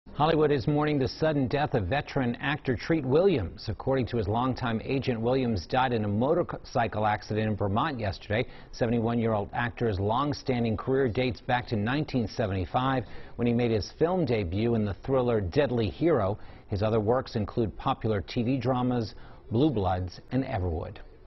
'Everwood' actor Treat Williams dies after motorcycle crash in Vermont
Actor Treat Williams, known for his role in "Everwood" has died after a motorcycle crash in Vermont. He as 71.